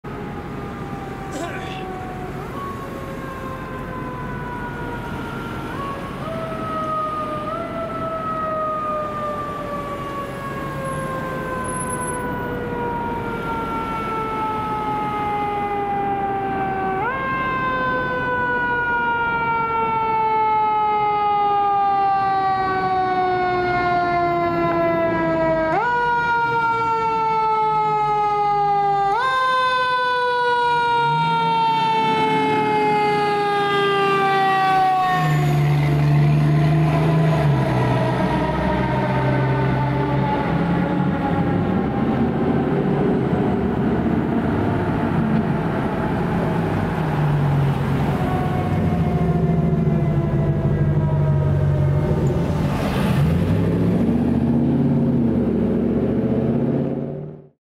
LAFD Engine 229 Responding
Los Angeles Fire Department Engine 229 (reserve engine) responding to a flooding on Oxford Avenue near San Marino Street.

Engine 229 (Ex-Engine 27, Ex-Engine 227) is a 2003 Pierce Arrow XT triple

Koreatown, Los Angeles, CA - 11/25/2022